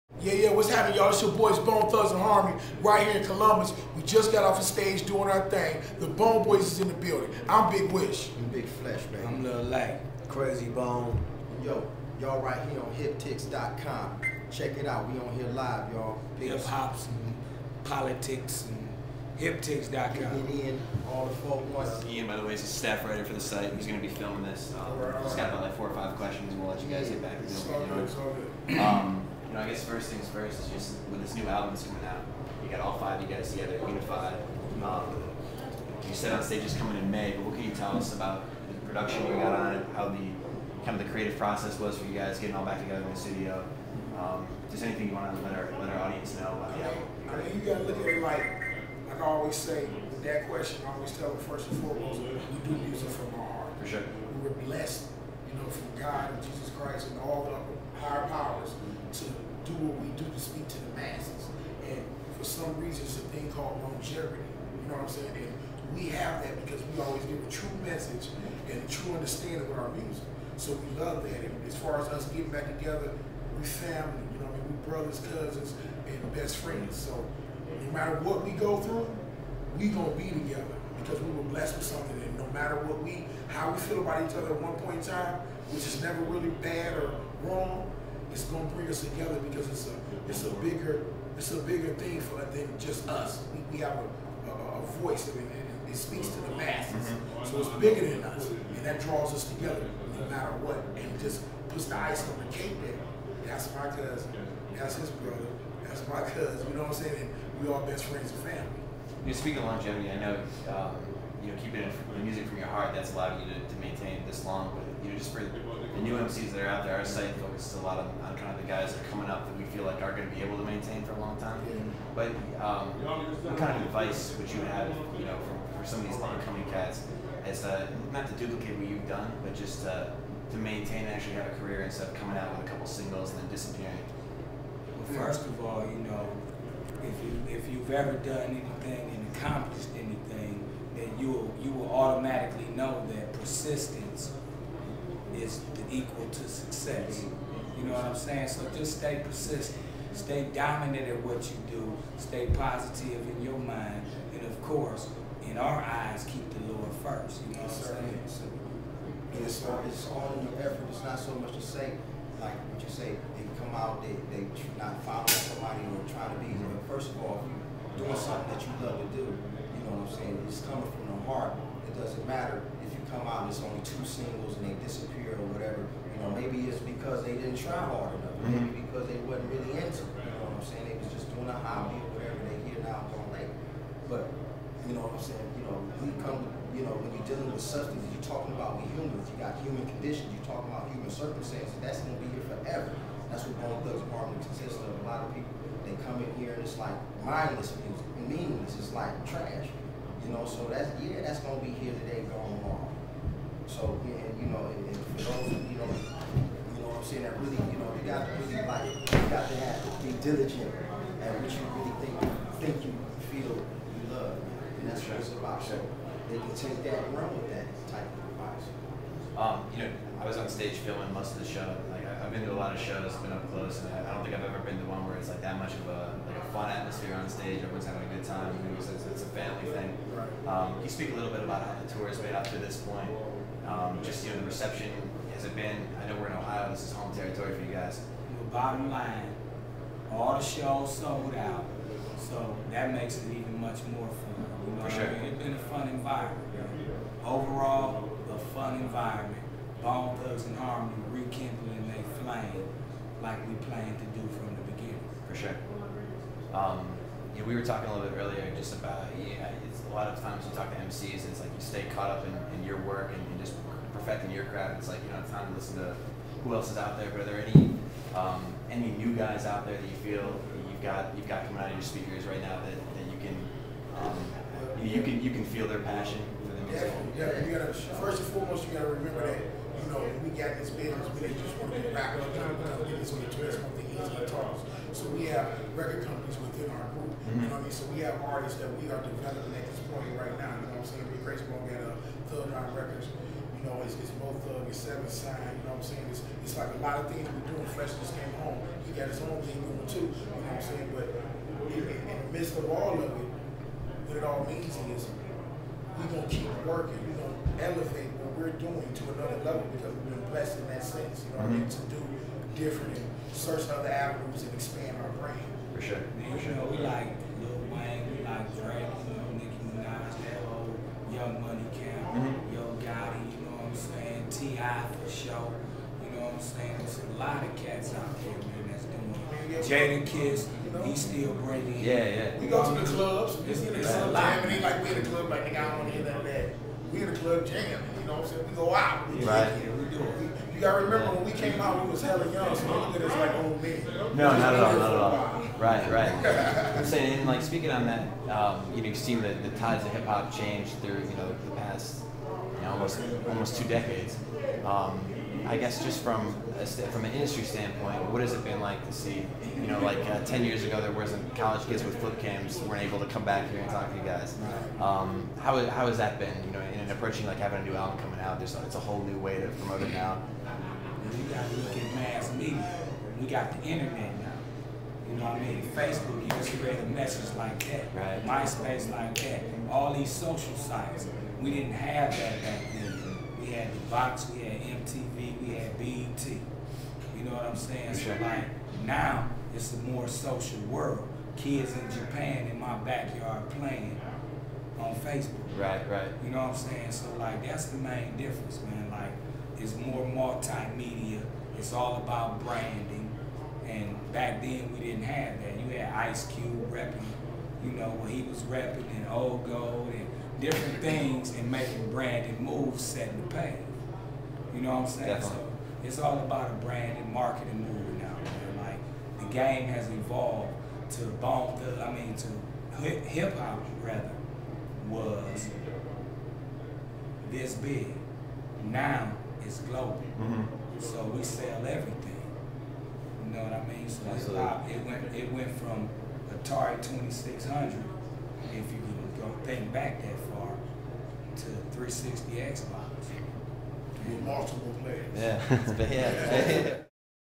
Bone Thugs-N-Harmony Interview 2010 Ohio
Bone Thugs-N-Harmony's HIPTICS.com Interview after sold-out show in Columbus, Ohio.